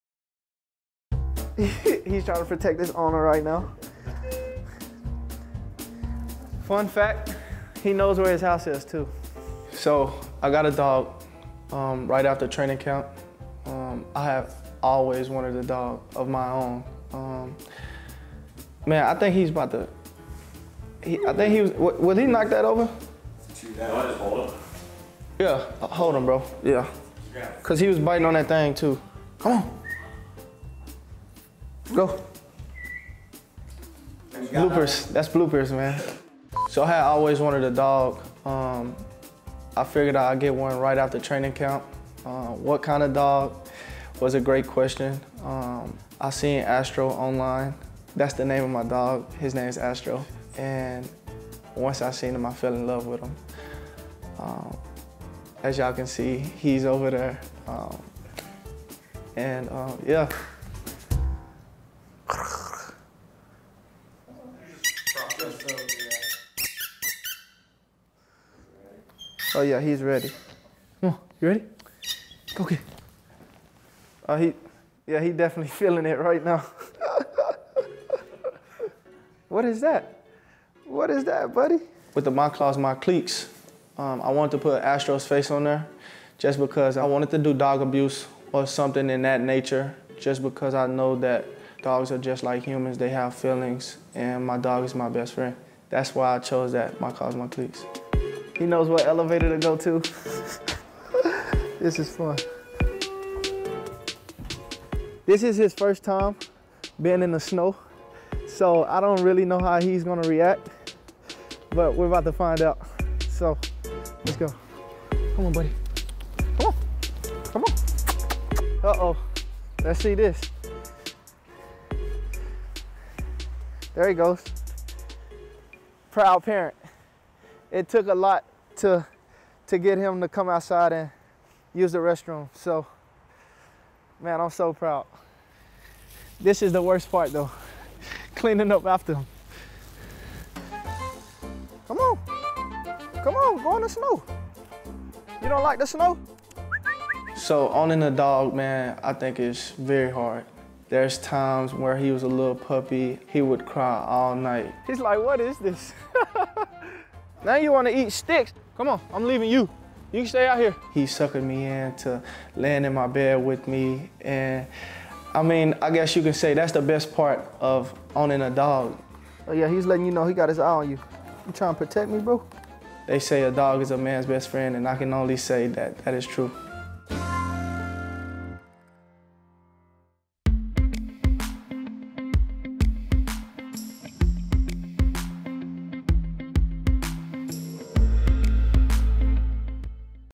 Rashard Higgins Adopts a New Puppy | Browns Player Spotlight
From a young age, Browns wide receiver Rashard Higgins has always wanted his own dog. Following the Brown’s 2018 Training Camp, Higgins adopted a French Bulldog puppy and named him Astro. Watch as he introduces the world to his new best friend!